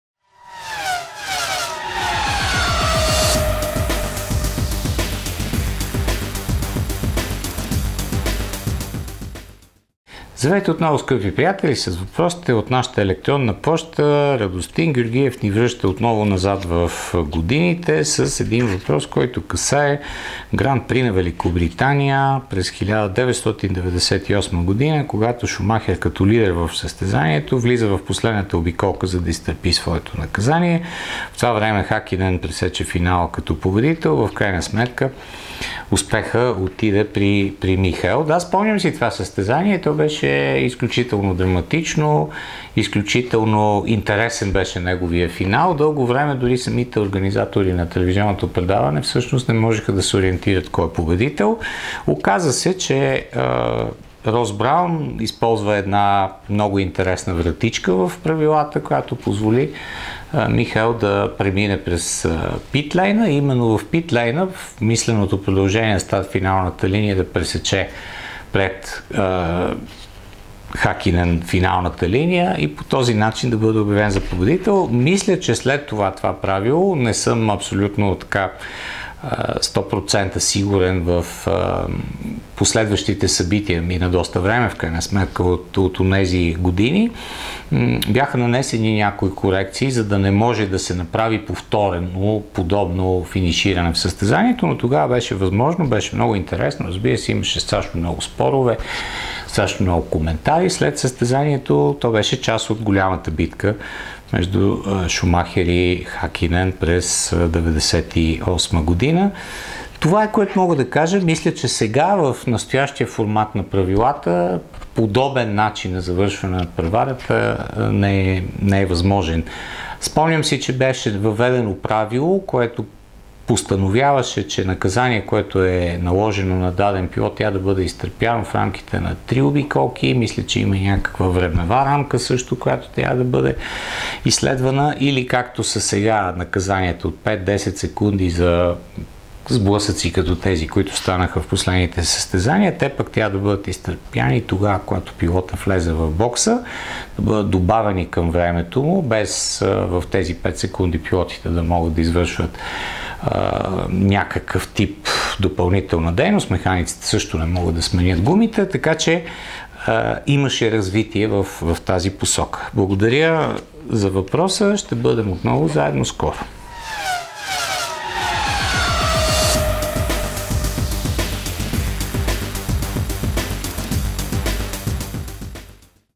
Спомени за победата на Шумахер в Гран при на Великобритания 1998
Коментарът ви за дъждовната и супер драматична Гран при на Великобритания през 1998 г., когато Шумахер, като лидер в състезанието, влезе в последната обиколка за изтърпяване на своето наказание,  а през това време Хакинен пресече финала на надпреварата и видя пръв карирания флаг.

Въпрос на Радостин Георгиев